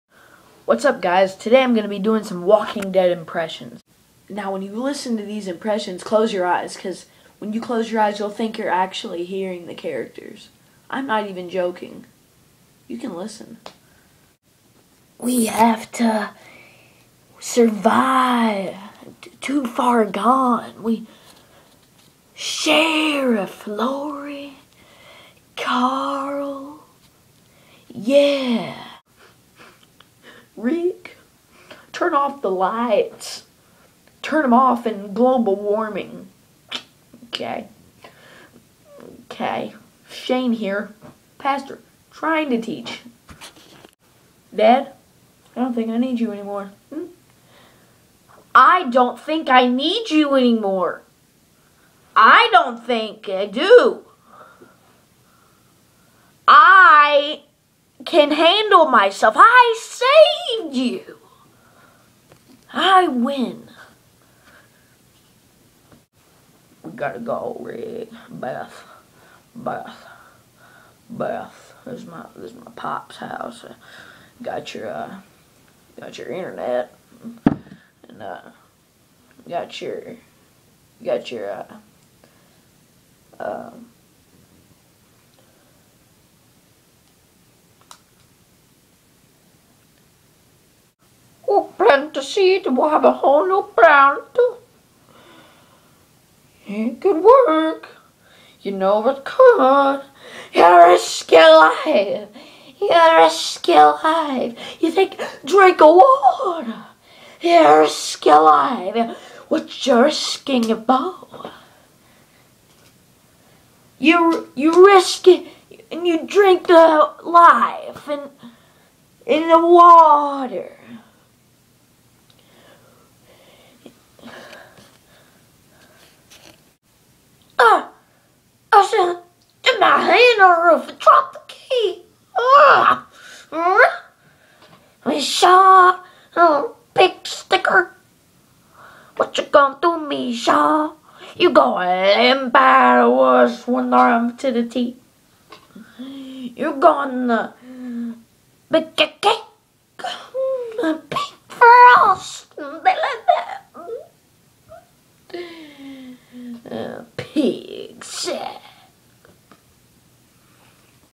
The Walking Dead Perfect Voice Impressions
Amazing impressions from Rick Daryl  Merle Carl Shane Hershel